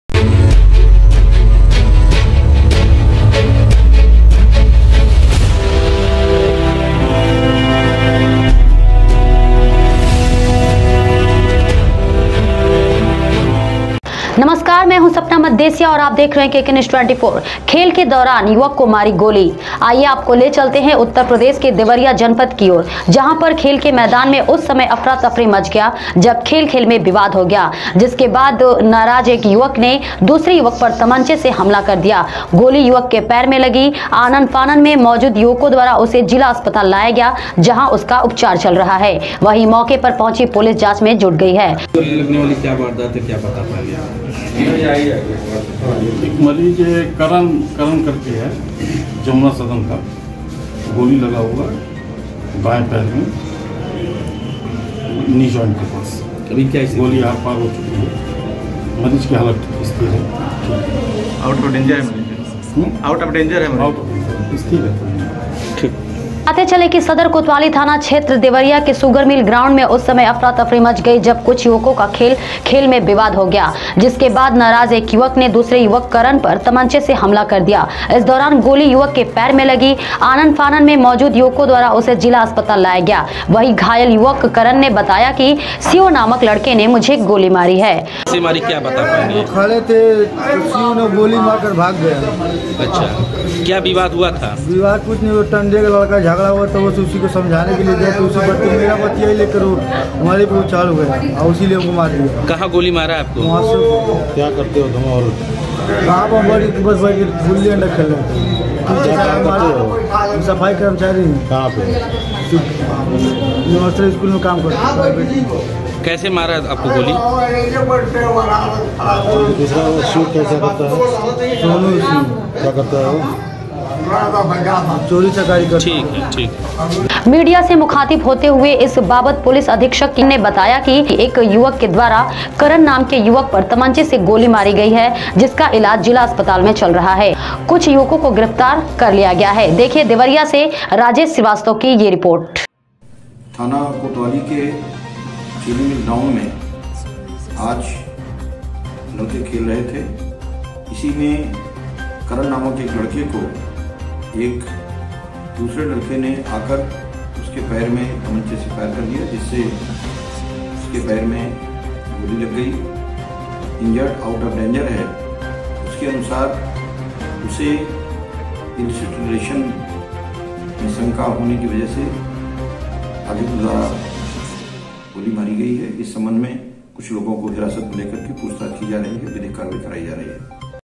kknews24 देवरिया :: खेल के दौरान युवक को मारी गोली
good covring news

देवरिया :: खेल के दौरान युवक को मारी गोली
    आइए आपको ले चलते हैं उत्तर प्रदेश के देवरिया जनपद की ओर जहां पर खेल के मैदान में उस समय अफरा तफरी मच गया जब खेल खेल में विवाद हो गया। जिसके बाद नाराज एक युवक ने दूसरे युवक पर तमंचे से हमला कर दिया, गोली युवक के पैर में लगी। आनन फानन में मौजूद युवकों द्वारा उसे जिला अस्पताल लाया गया जहां उसका उपचार चल रहा है। वही मौके पर पहुँची पुलिस जांच में जुट गई।
        बताते चलें की सदर कोतवाली क्षेत्र देवरिया के शुगर मिल ग्राउंड में उस समय अफरा तफरी मच गई कुछ युवकों का खेल खेल में विवाद हो गया। जिसके बाद नाराज एक युवक ने दूसरे युवक करन पर तमंचे से हमला कर दिया। इस दौरान गोली युवक के पैर में लगी। आनन फानन में मौजूद युवकों द्वारा उसे जिला अस्पताल लाया गया। वहीं घायल युवक करन ने बताया कि शिव नामक लड़के ने मुझे गोली मारी है।
मीडिया से मुखातिब होते हुए इस बाबत पुलिस अधीक्षक कि एक युवक के द्वारा करन नाम के युवक पर तमंचे से गोली मारी गई है जिसका इलाज जिला अस्पताल में चल रहा है। कुछ युवकों को गिरफ्तार किया गया है। देखिए देवरिया से राजेश श्रीवास्तव की ये रिपोर्ट.
बाइट- करण(घायल युवक)
बाइट- डॉ श्रीपति मिश्र (एसपी)